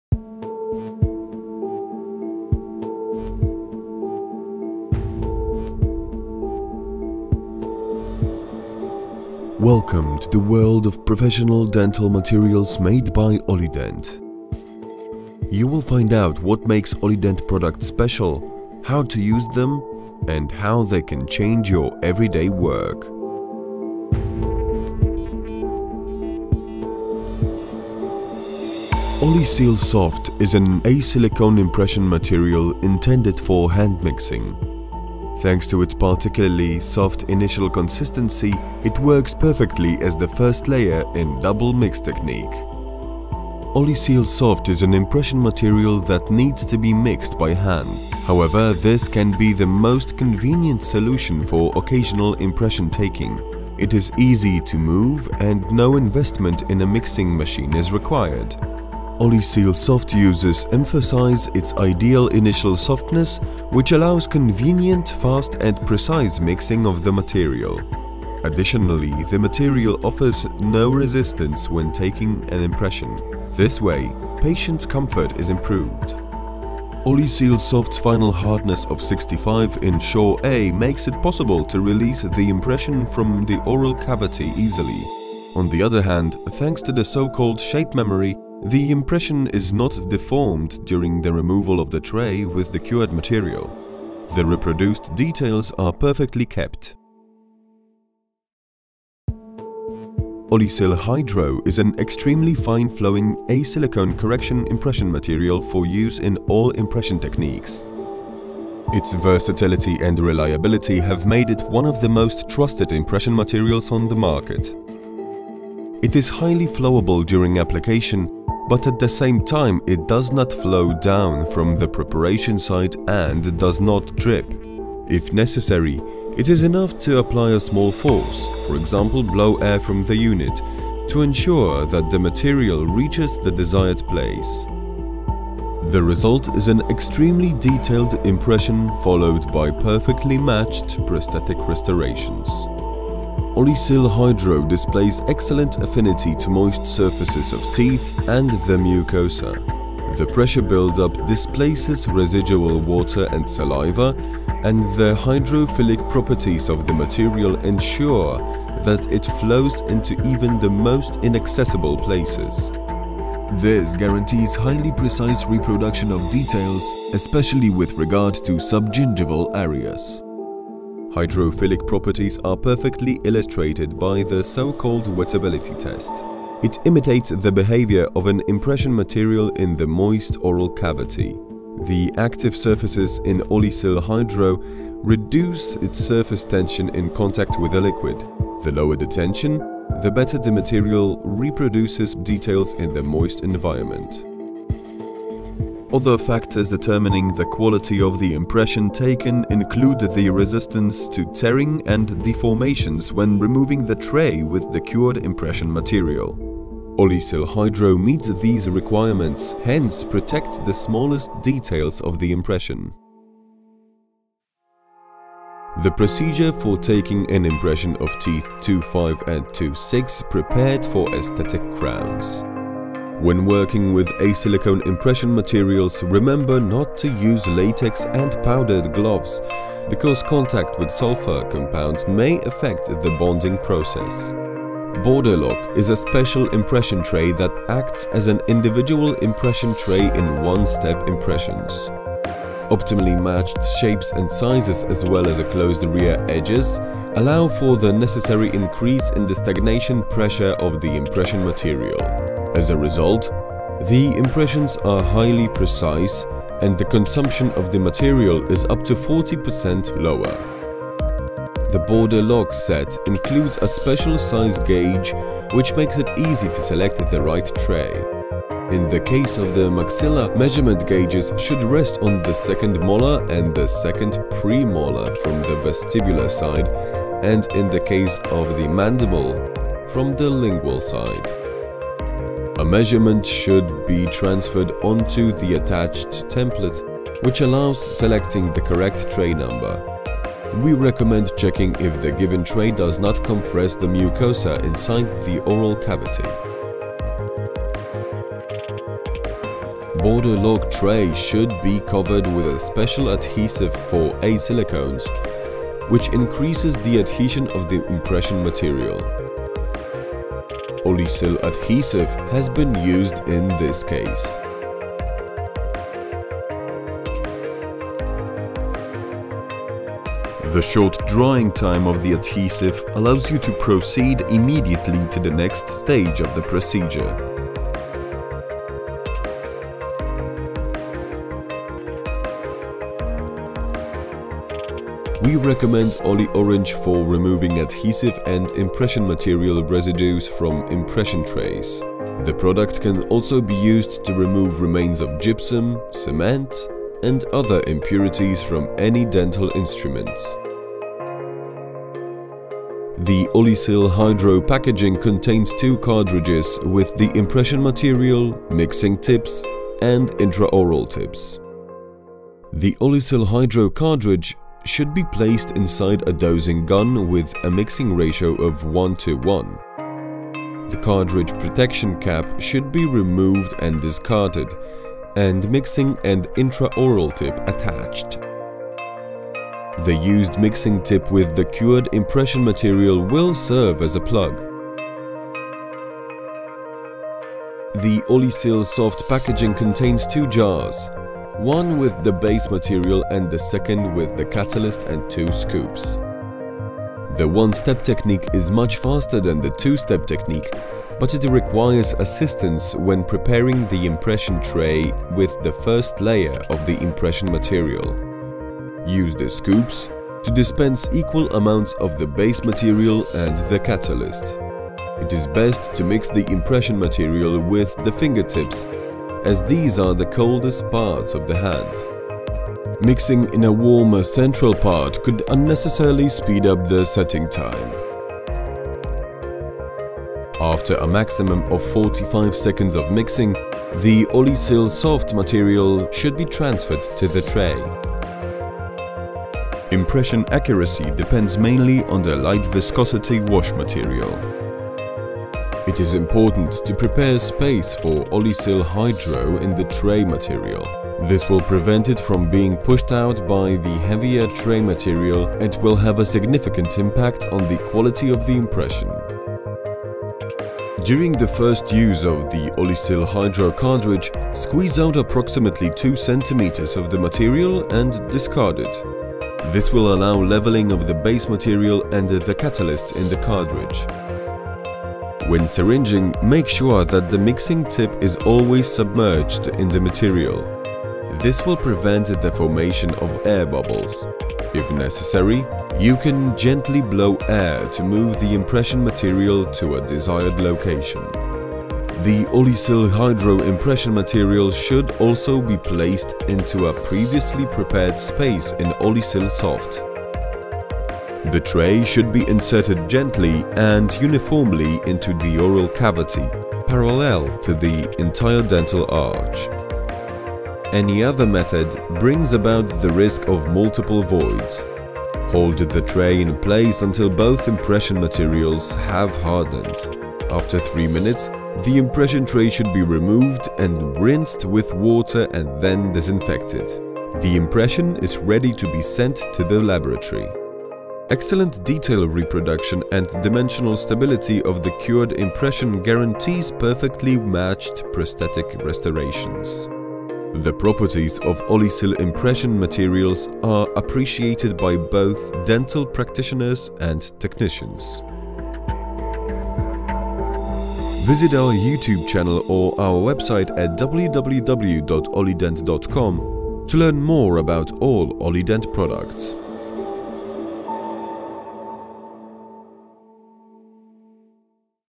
OliSil SOFT & HYDRO - OLIDENT [EN]
OliSil SOFT is especially soft impression material, based on additive silicones. Because of its consistency, it's perfect for the double-mix technique. Impressions made with this technique are of highest precision and no internal stress.